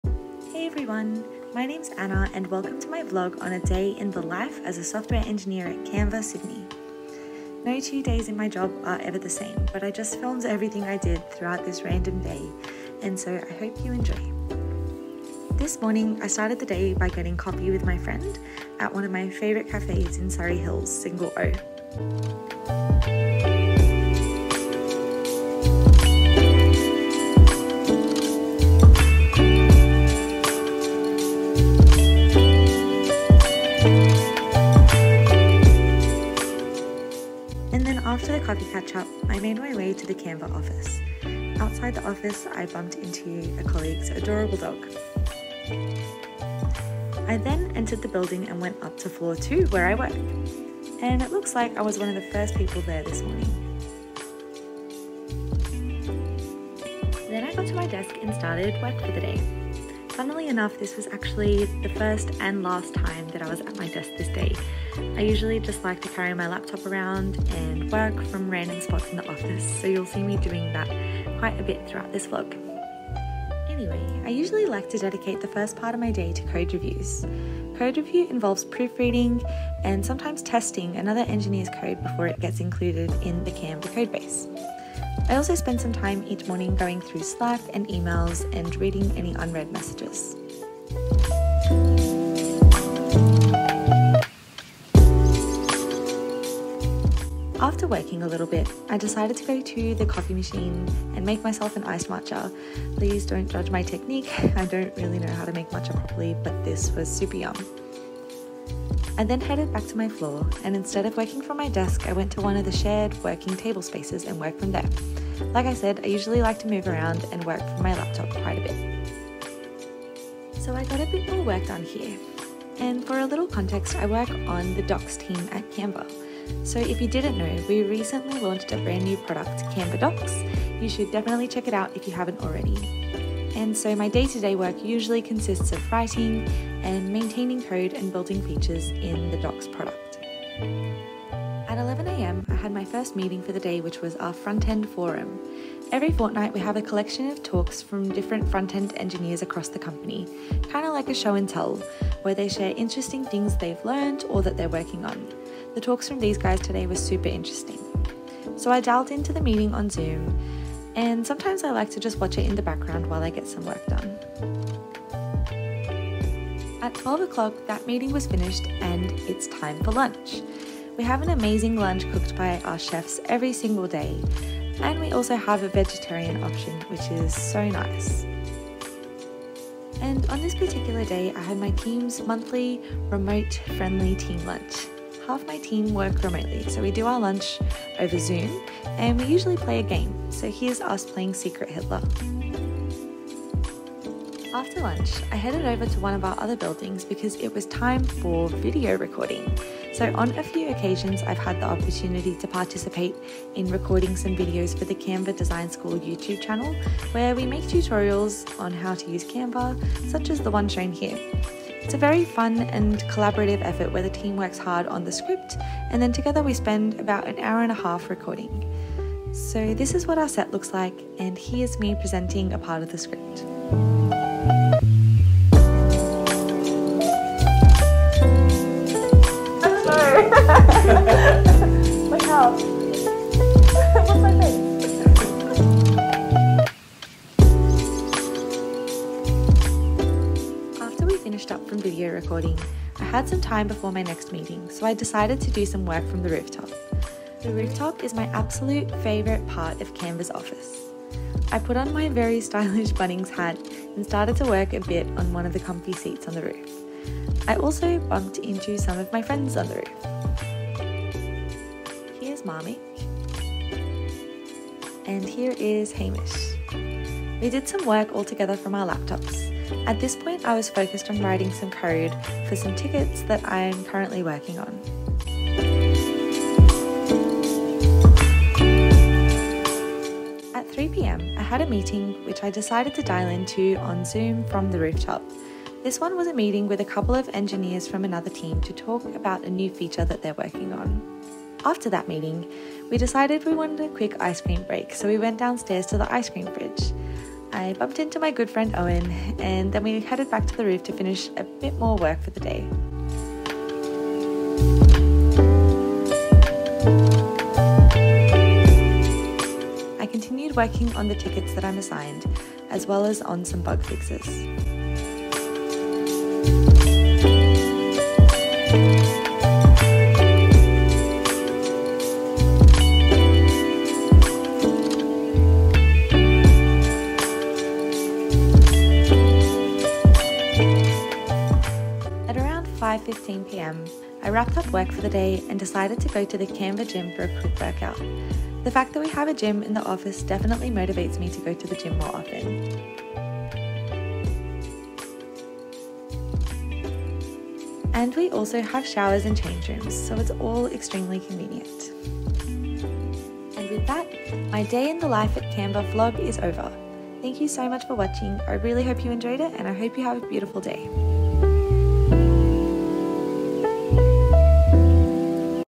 Day in the life of a Canva Software Engineer | Sydney
See what I get up to in a day at the Sydney Canva office as a Software Engineer!